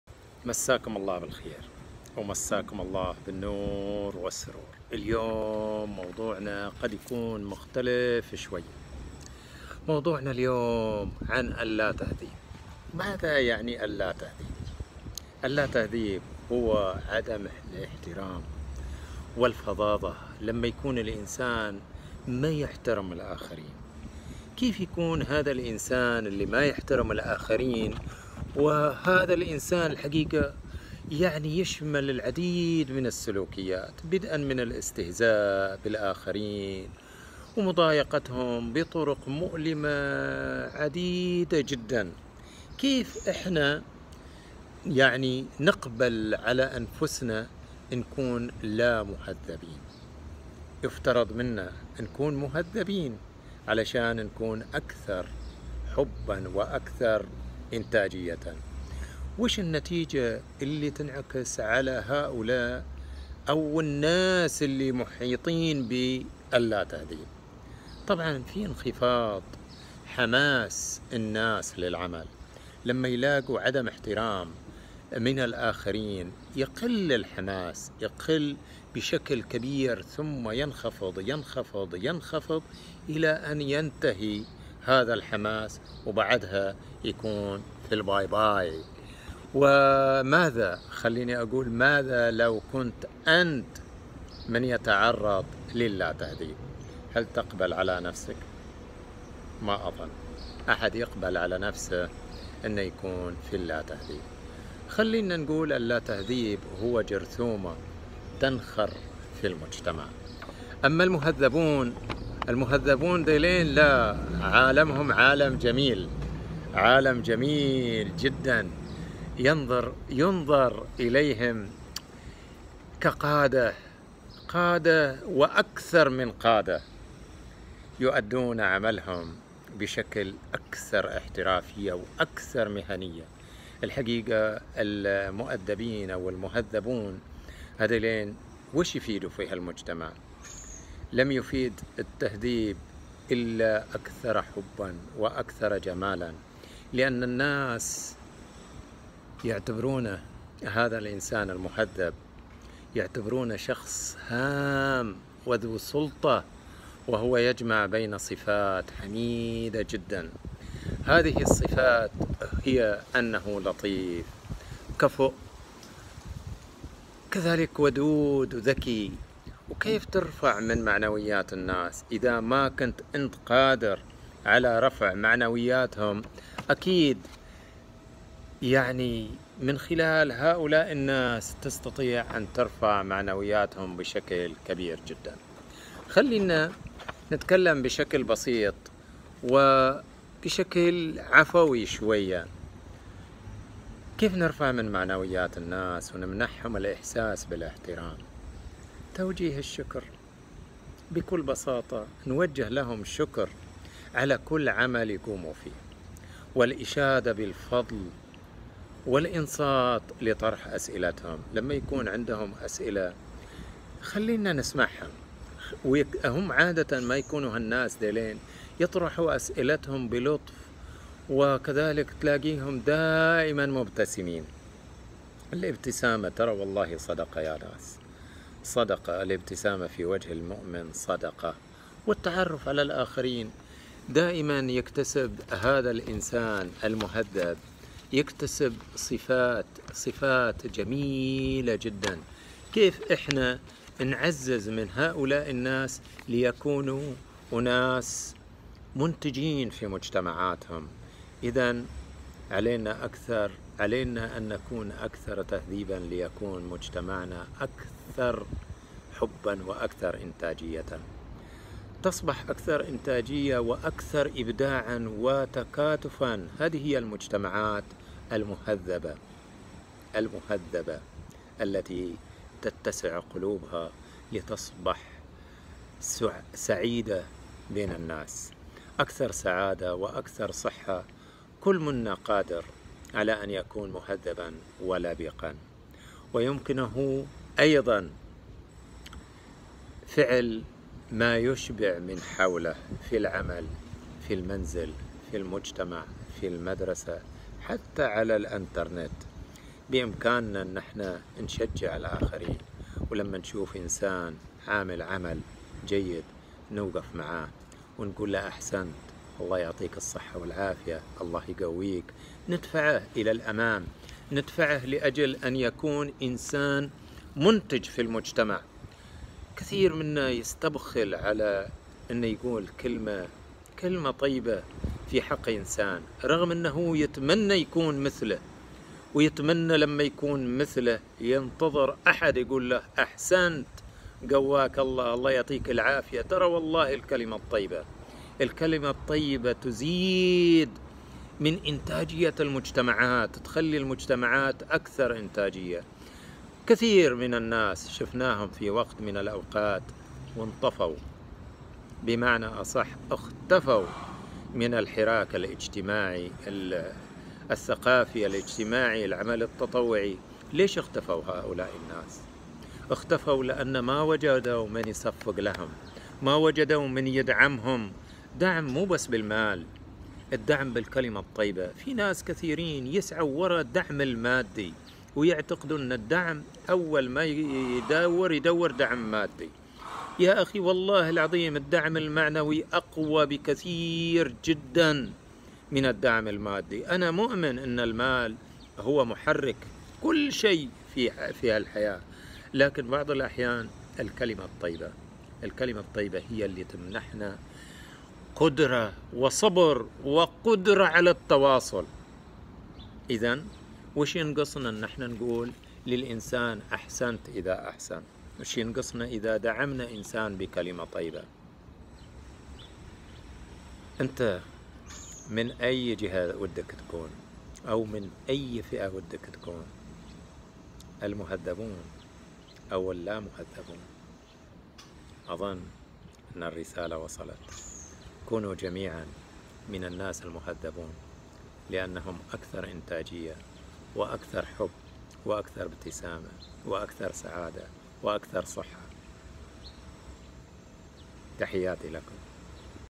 المهذبين أكثر إنتاجية وسعادة
اللا تهديب هو عدم الإحترام والفظاظة
المهذبون هم القادة الأشخاص الأكثر سعادة
ملتقى المحبة والسلام